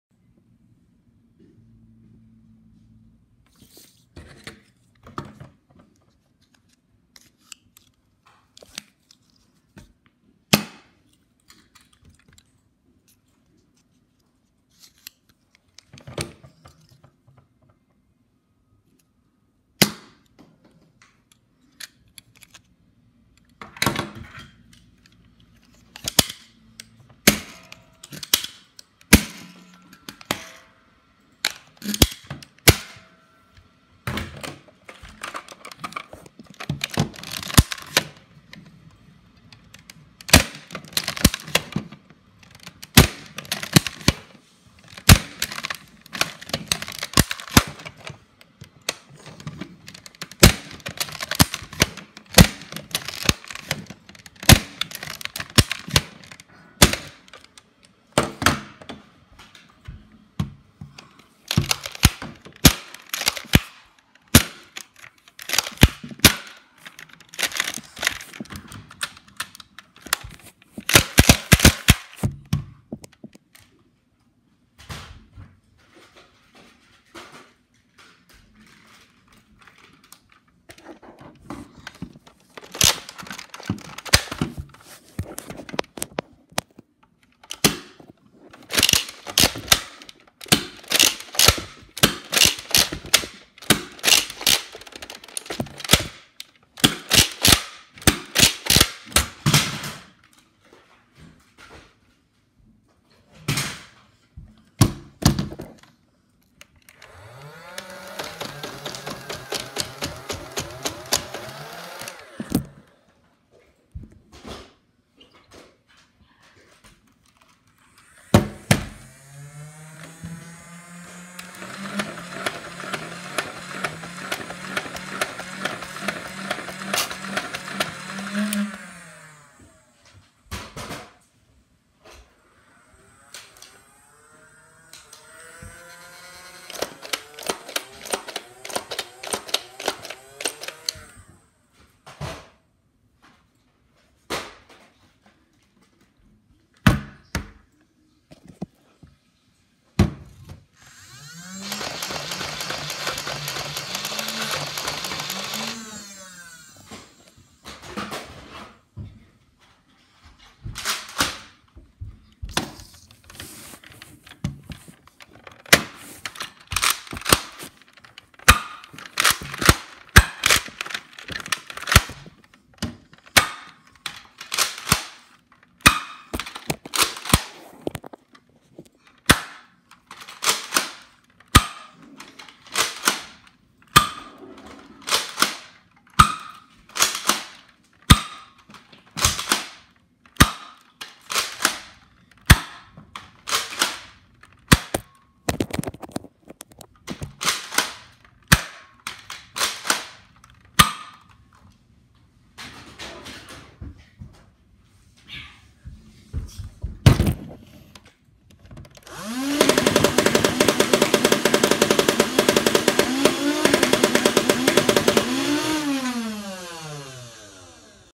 Shooting all my nerf guns
The unseen one shooting Elite 2.0 darts is the rapidstrike. (Not all nerf guns included in this video)